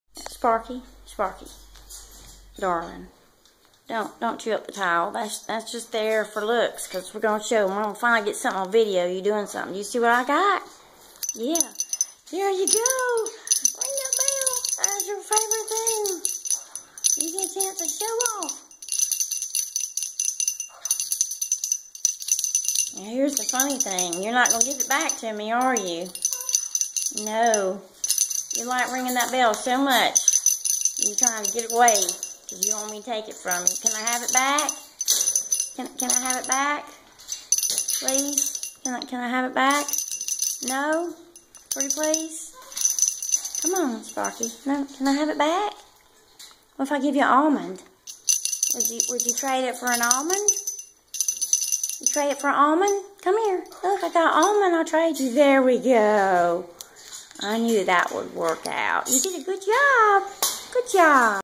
Parrot ringing bell
"Sparky" my Timneh African Grey doing one of his favorite things.  He loves to ring any kind of bell.  Always under my supervision